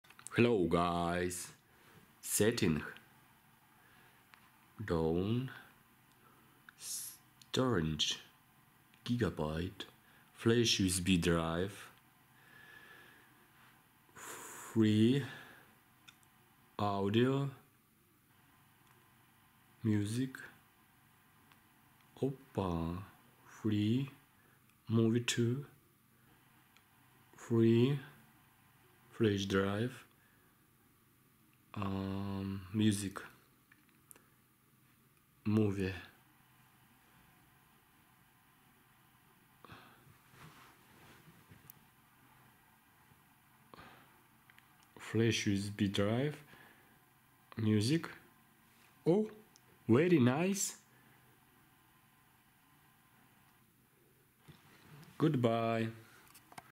How to transfer music from phone to flash drive
How to download music from phone to flash drive?